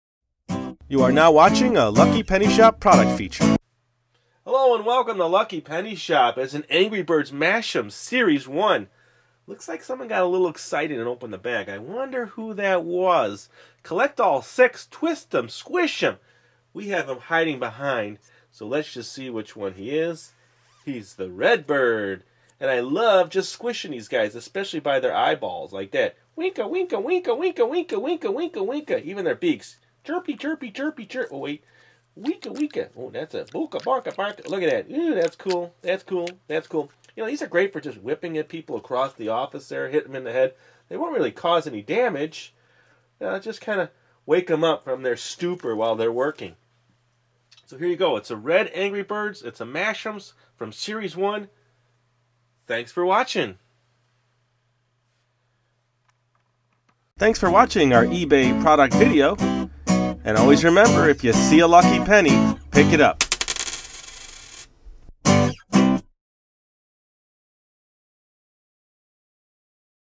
Angry Birds Mashems Series 1 Red Bird Mystery Foil Packs by Tech4Kids - Twist, Squish & Launch 'em
​​​Watch our product feature video for Angry Birds Mashems Red Bird in Foil Packs by Tech4Kids.